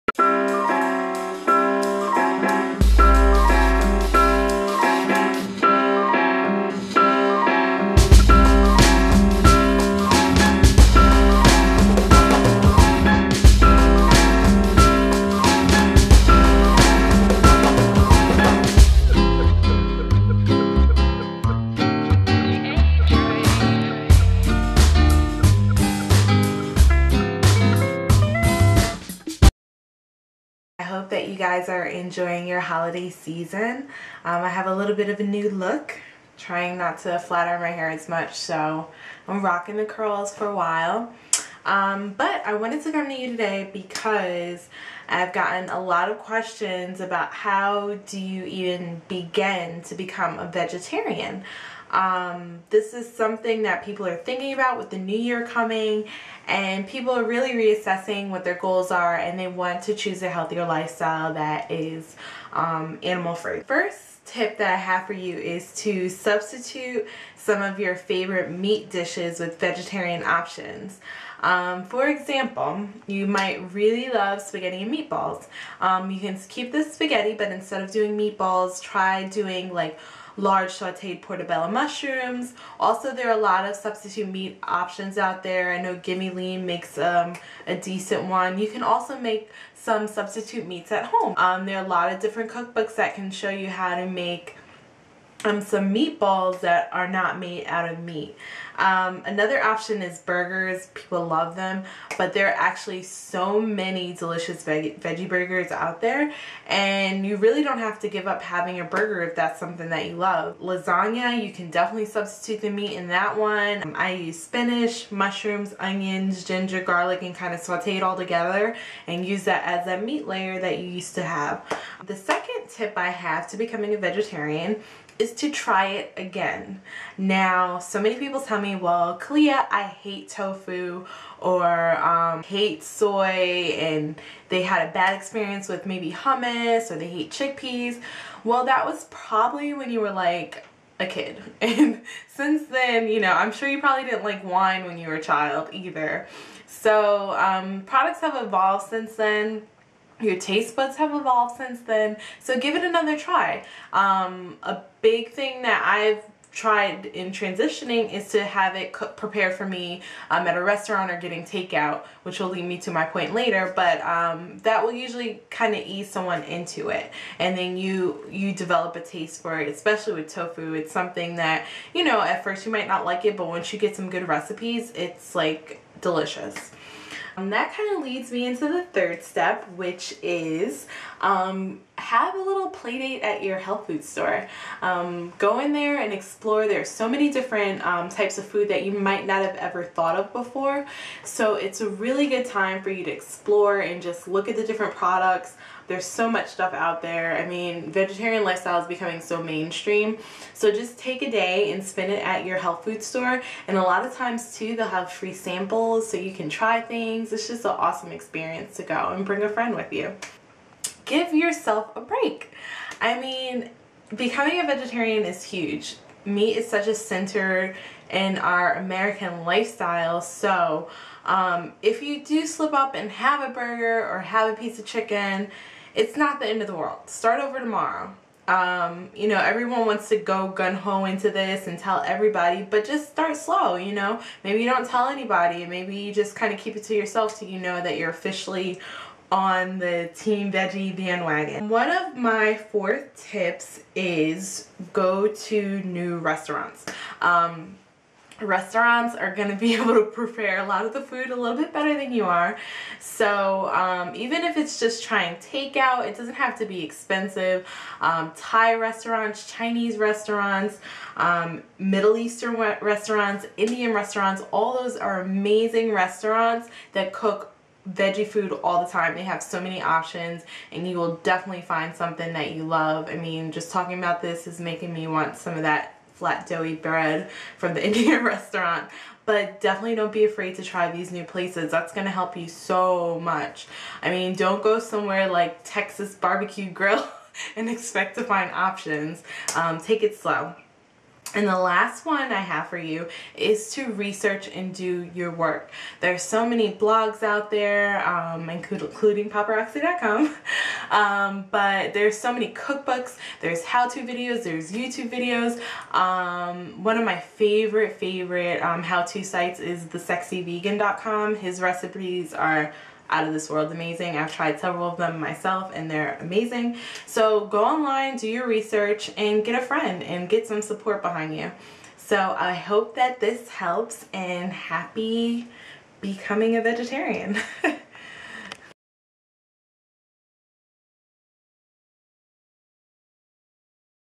How to become a vegetarian?
's Roxi discusses how to become a Vegetarian.